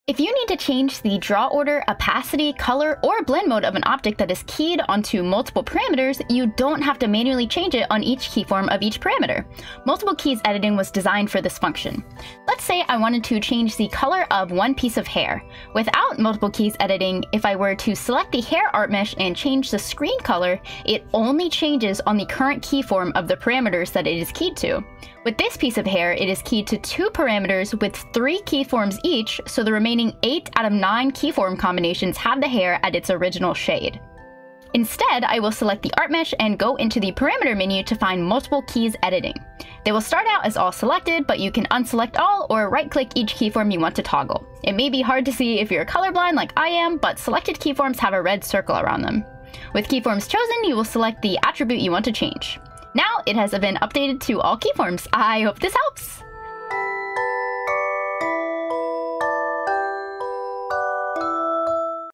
Live2D Quick Tip - Multiple Keys Editing
It took me way too long editing draw order one-by-one before I learned about Multiple Keys Editing in Live2D! I hope I can save you from the same fate!

Model Rigger (WIP): CraftyKaname

0:00 Intro
0:12 Example of Issue
0:34 How to Use Multiple Keys Editing

Catch me stream live on Twitch!